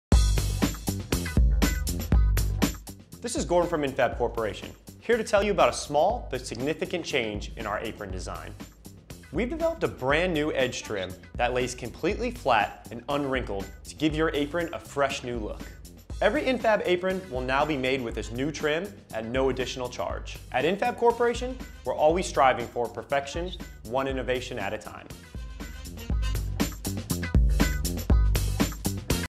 Infab Lead Apron Trim
Infab introduces the latest lead apron trim technology, providing a flat look and no wrinkles.